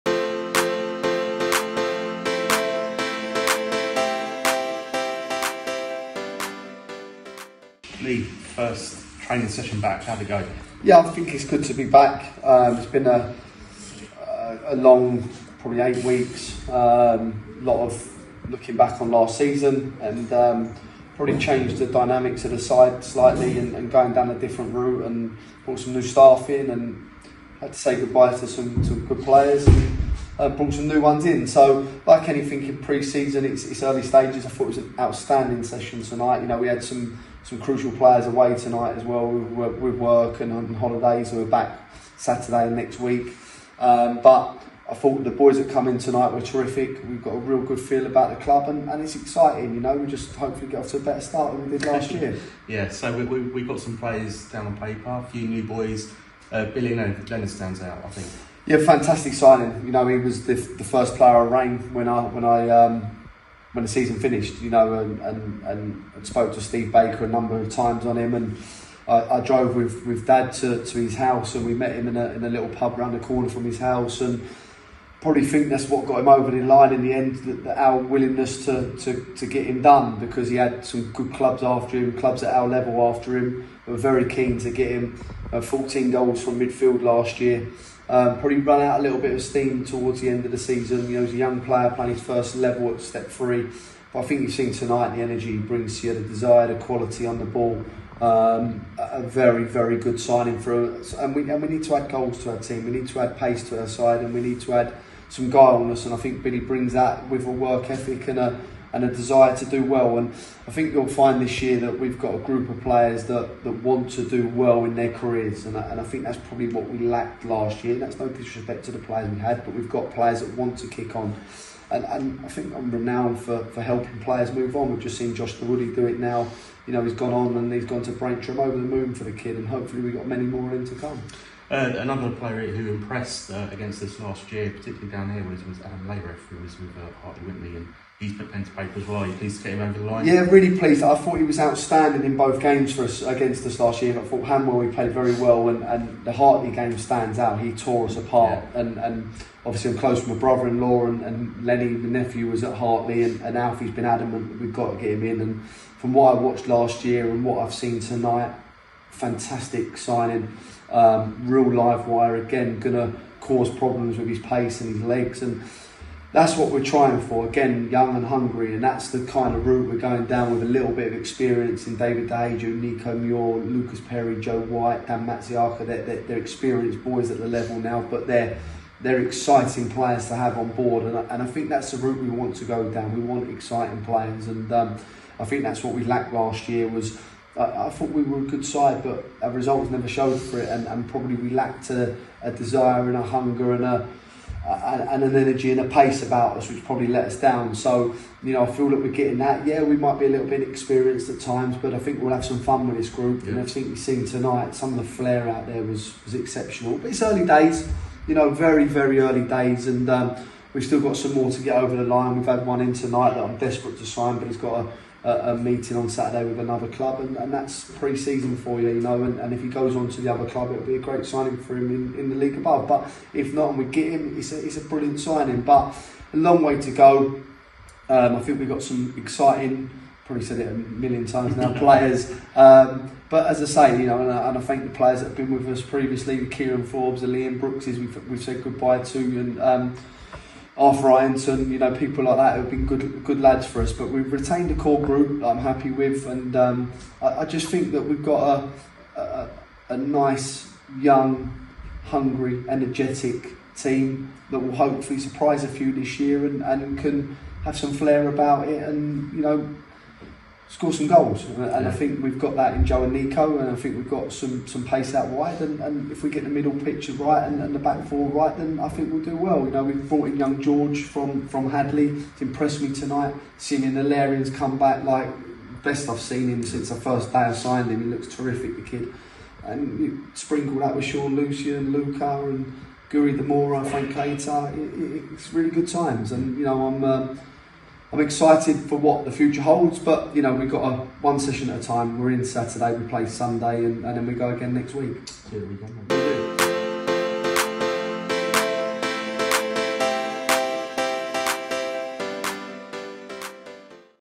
LEE ALLINSON - First day back!
Lee speaks with John after the first day back for pre-season.